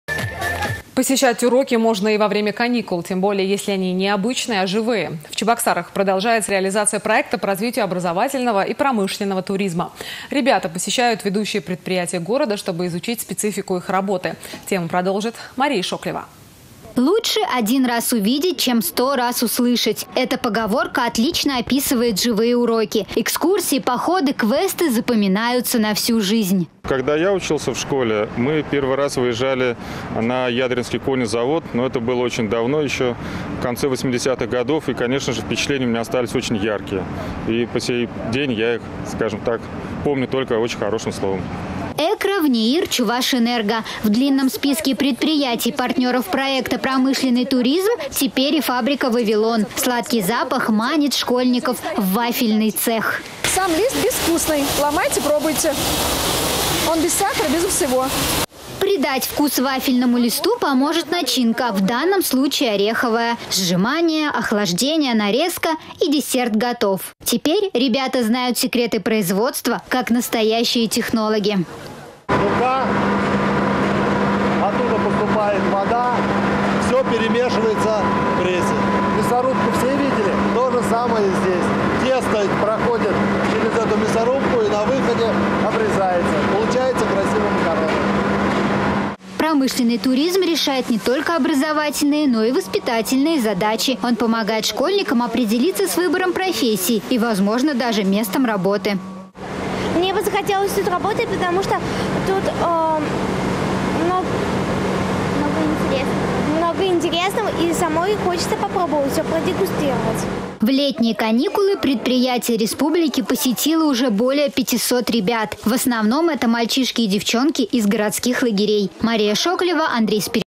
В Чебоксарах продолжается реализация проекта по развития образовательного и промышленного туризма
Посещать уроки можно и во время каникул! Тем более, если они не обычные, а живые. В Чебоксарах продолжается реализация проекта по развития образовательного и промышленного  туризма.